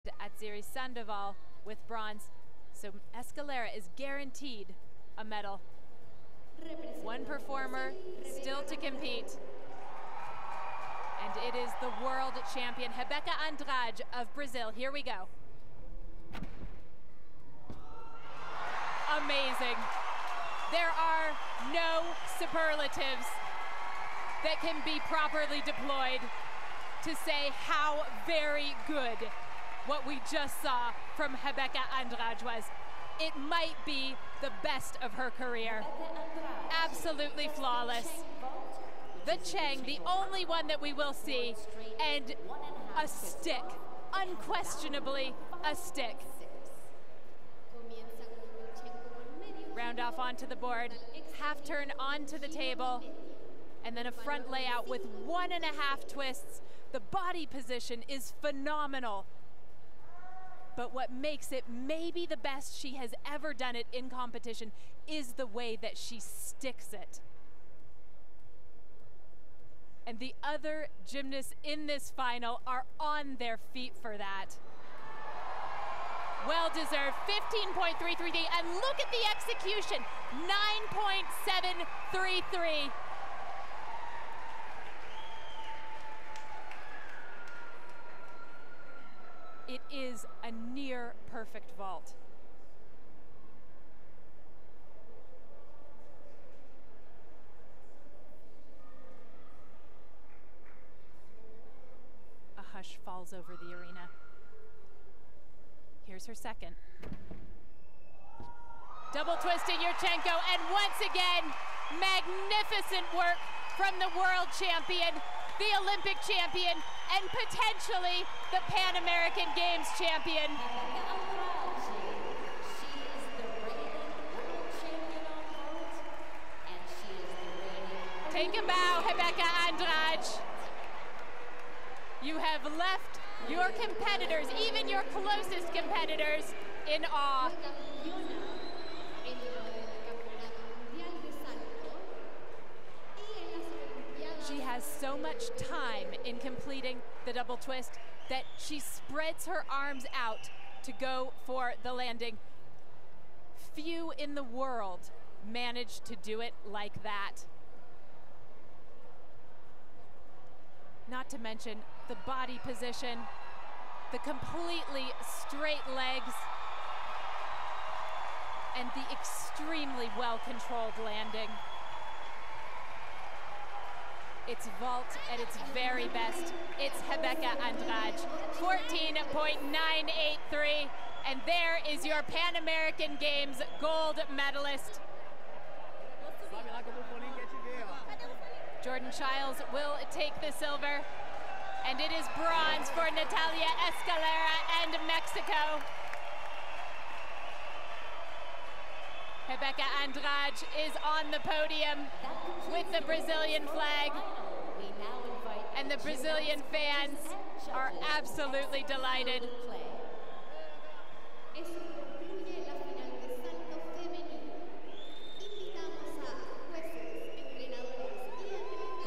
Rebeca Andrade (BRA) - Vault - 2023 Pan American Games Women's Gymnastics Event Finals
1st place, gold medal. Rebeca Rodrigues de Andrade captures Brazil's first gymnastics gold medal of the 2023 Pan American Games.

2023 Pan Ams WAG EF - Vault Final
'23 Pan Am Games Santiago Chile
Brasil Gym Jogos Pan-Americanos
Ginástica artística